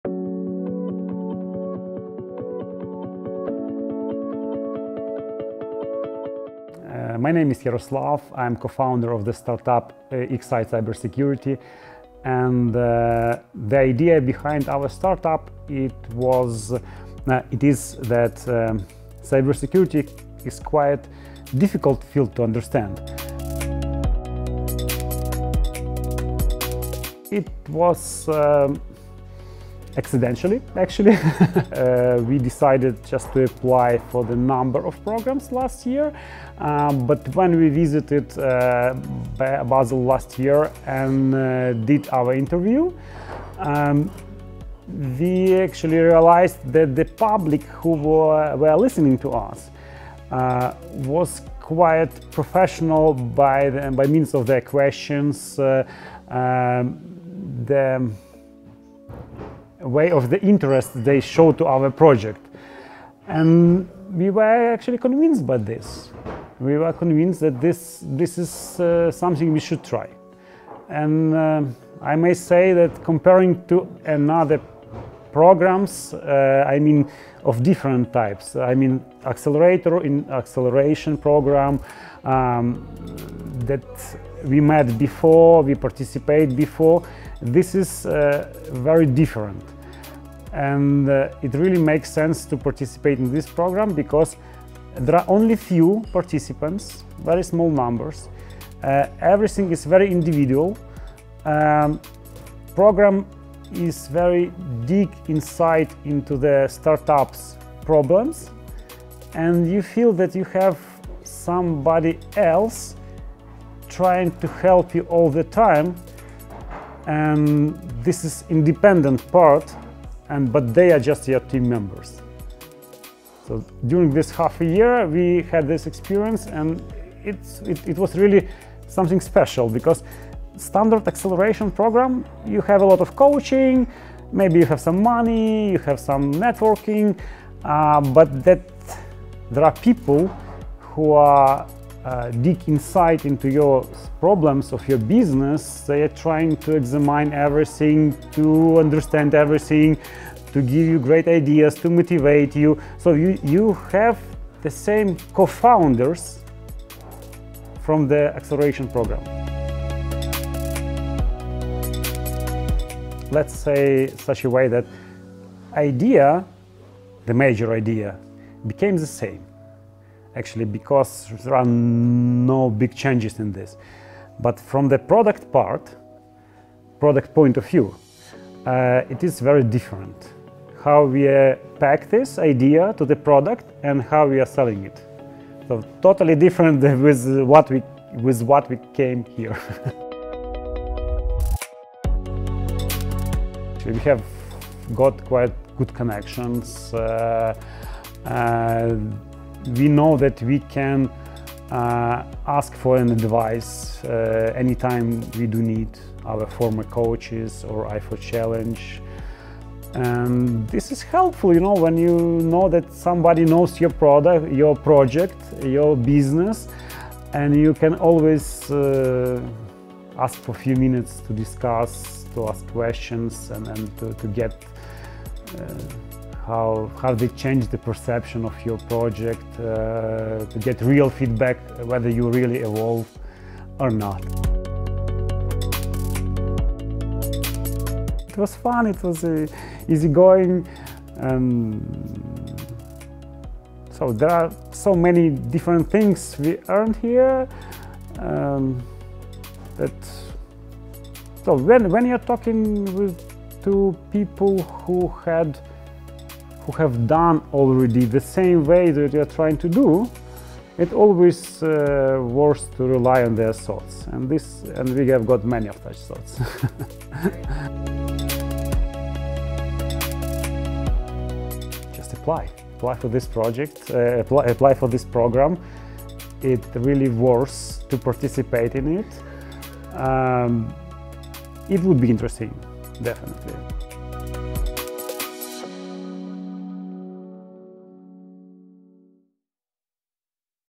i4Challenge New Ideas winner 2022/2023 - Yaroslav Pryymak
We’re proud to present the five phenomenal i4Challenge New Ideas winners who showcase the innovation potential of the region in its full effect. Six months ago, they started with an idea. Today, they have MVPs, and some even have clients and investors lined up.

Xsight is developing an offensive AI technology to continuously monitor and identify cybersecurity vulnerabilities. This proactive approach aims to significantly reduce the time it takes companies to discover they've been hacked, thereby minimizing associated losses.

Yaroslav Pryymak, co-founder of Xsight, applied to numerous acceleration programs. The professional and keen interest shown during his application process at the i4Challenge convinced him to give it a shot. Yaroslav was impressed by the individualized approach. With only a handful of participants, the program delved deep into the startups' issues, making the mentors feel like an extension of the team, almost like co-founders.

The core idea behind Xsight remains the same, but the way it’s packed into a product drastically improved. Additionally, Yaroslav forged valuable connections that are always available for discussions, providing real, actionable feedback even after the i4Challenge accelerator concluded.

His advice to anyone who considers applying?
“Just apply. It really is worth it to participate.”